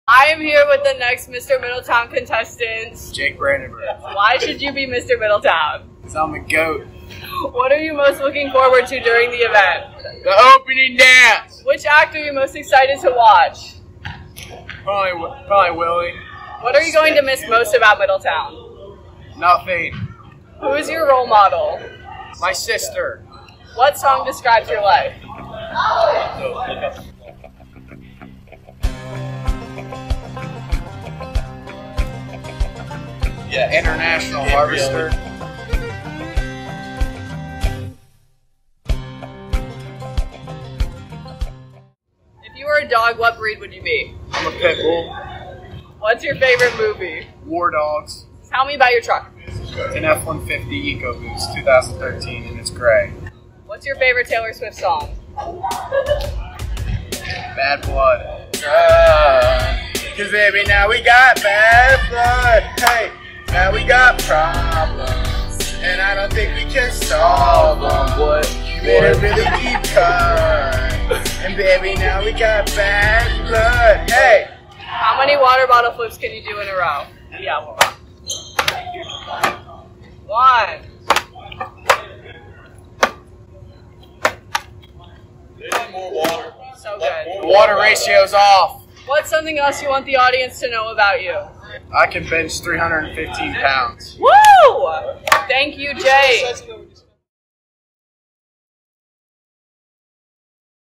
RT+: Mr. Middletown contestant, Jake Brandenburg
Meet Mr. Middletown contestant, Jake Brandenburg. The event is Saturday, May 14 in the MHS auditorium.